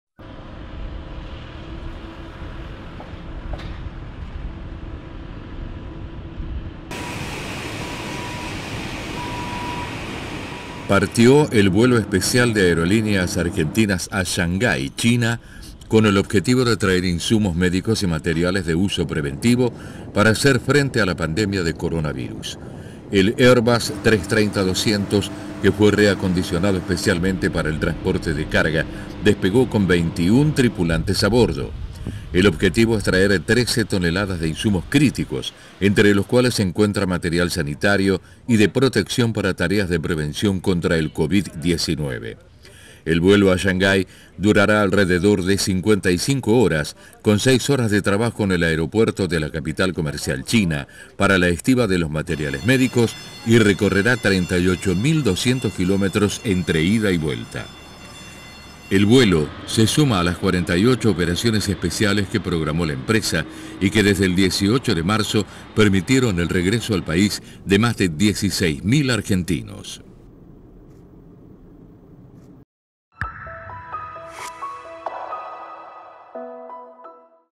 Partió el vuelo a China para traer 13 toneladas de insumos médicos
Partió el vuelo especial de Aerolíneas Argentinas a Shanghái, China, con el objetivo de traer insumos médicos y materiales de uso preventivo para hacer frente a la pandemia de coronavirus y que se suma a otros coordinados por Cancillería, con el mismo fin, desde el gigante asiático.